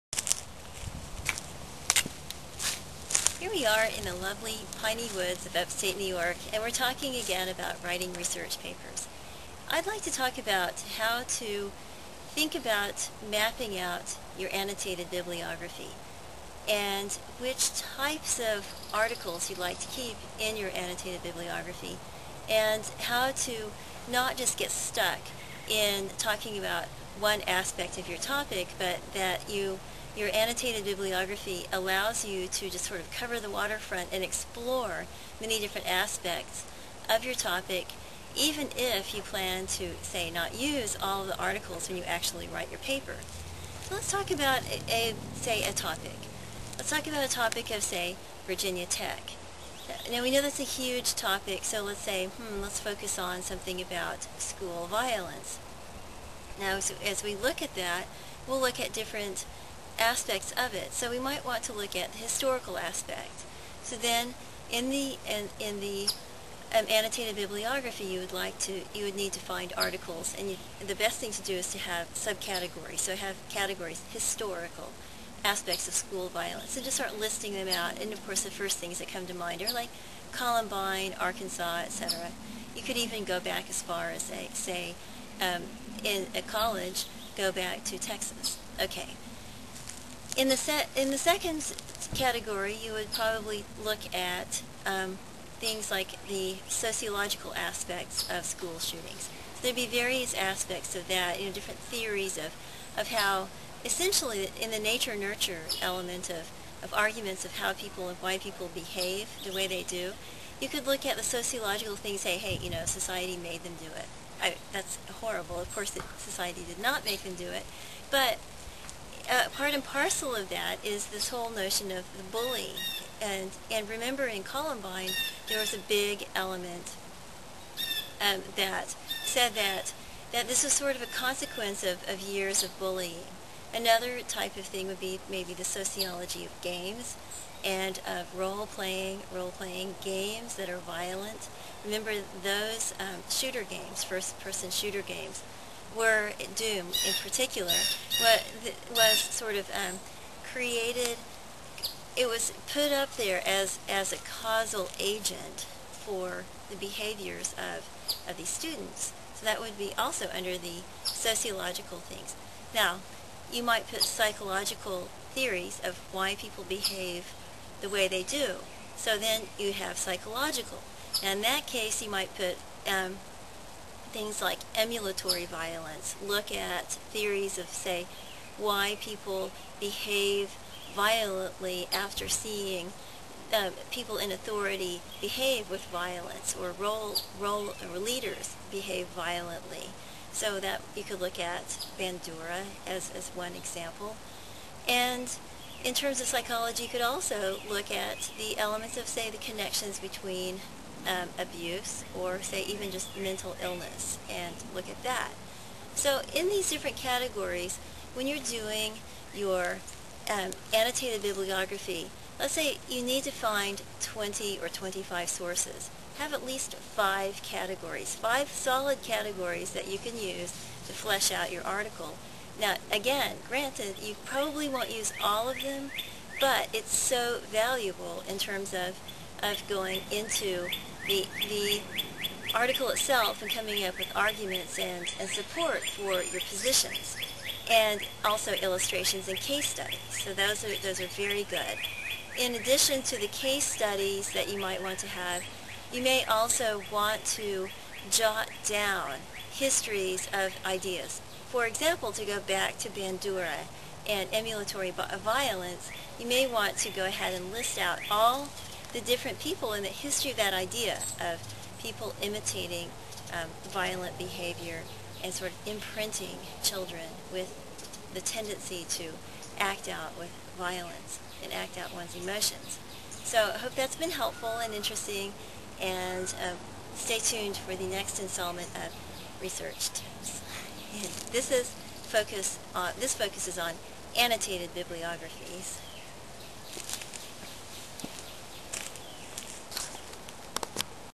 Constructing an Effective Annotated Bibliography: Virginia Tech example
This video discusses how to develop an effective annotated bibliography. Included is a step-by-step approach applied to the example of a paper that is investigating school violence, and the examples of Virginia Tech, Columbine, and theories of emulatory violence (Bandura), sociological pressures, psychological consequences of abuse, etc. How to develop categories for your annotated bibliography is discussed. Susan Smith Nash, Ph.D. is your guide.
•      Bullet Points: annotated bib, investigating violence, developing categories, creating a strong bib, psychological consequences